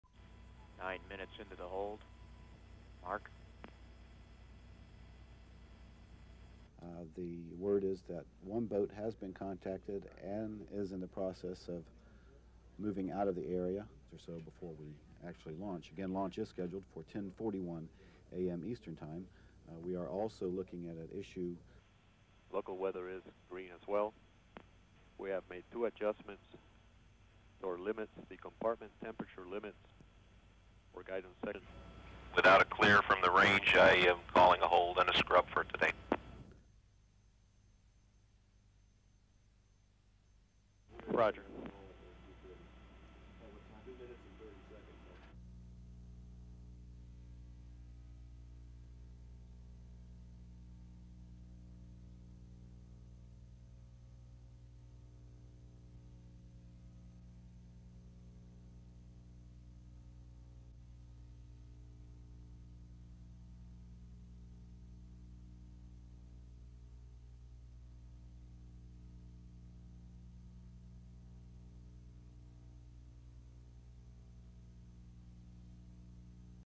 USA: NASA ROCKET LAUNCH CANCELLED AS SHRIMP BOATS GET TOO CLOSE
A NASA rocket carrying a robotic explorer was all set to lift off for deep space on Sunday -- until two wayward shrimp boats got too close to the launch site.

The boats had crossed into the zone where NASA recovers spent rocket motors that fall back to Earth.

The Delta rocket  was supposed to deploy the Advanced Composite Explorer, which will probe the chemical makeup of space and give advanced warnings of cosmic weather conditions that may effect Earth.

The Boeing-built Delta II rocket was ready to blast off from Florida on Sunday as the NASA prepared to deploy a probe to study space particles.

But trouble struck when a pair of 40-foot shrimp boats were detected in the waters near the launch site.

The boats were anchored in the danger zone off Cape Canaveral where the Delta's solid rocket boosters would fall after blastoff.

The Coast Guard contacted one of the boats, which moved out of the area.

The other boat would not respond to Coast Guard calls.

Fearing one of the nine boosters might hit the trawler, launch officials cancelled the liftoff.

SOUNDBITE: (English)
"Without a clear from the range I am calling a hold and a scrub for the day."
SUPER CAPTION: Rich Murphy, Flight Director

A spokesman at Cape Canaveral said scrubbing Sunday's launch would cost NASA at least 100-thousand dollars (US).

They will try again Monday, when -- they hope -- the area is clear of obstacles.

Once the 200-million (m) dollar (US) project is underway, scientists hope to learn more about the composition of the universe as the Advanced Composite Explorer, or ACE, sniffs its way through the cosmos.

ACE will also measure cosmic rays, solar wind and other space "weather" conditions that could affect the Earth, like magnetic fluctuations that can overload power grids or hamper astronauts working in space.

In all, there are nine experiments on board ACE.

Once free of Earth, four solar panels will unfold like petals, giving the cosmic explorer electricity to conduct its studies.

ACE will fly nearly 1 million miles (1.5 million km) from Earth to escape the planet's magnetic field, pointing its array of instruments toward the sun.